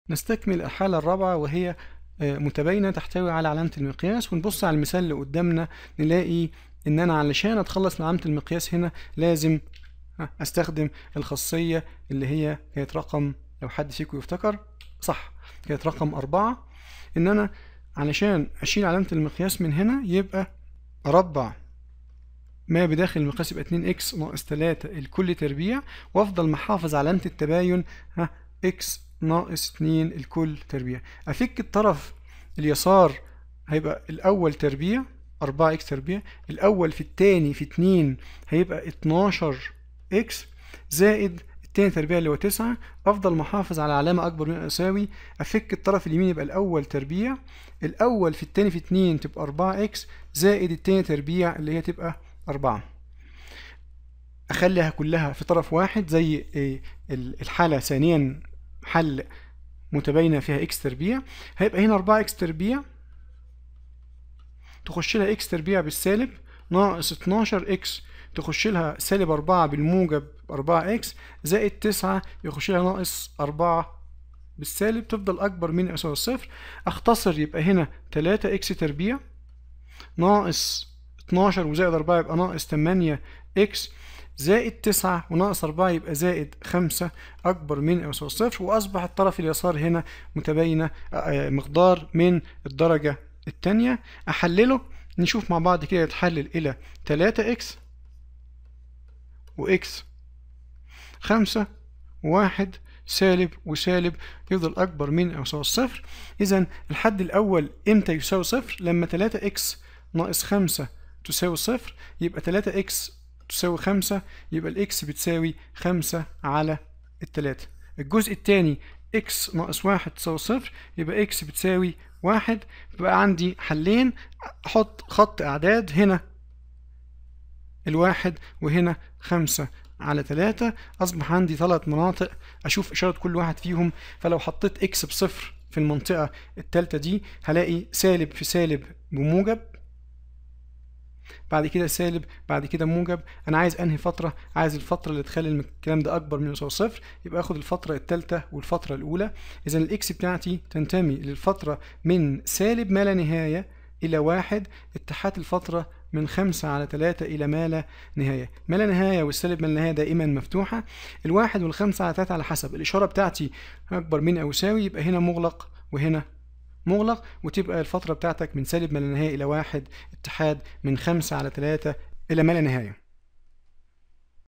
حساب التفاضل والتكامل - الوحدة 1 : المتباينات - 13 - Inequale
المقررات المفتوحة
الوحدة الأولى - مراجعة لما سوف نحتاجه قبل التفاضل والتكامل
مثال علي حل متباينة تحتوي علي دالة المقياس في متغير واحد.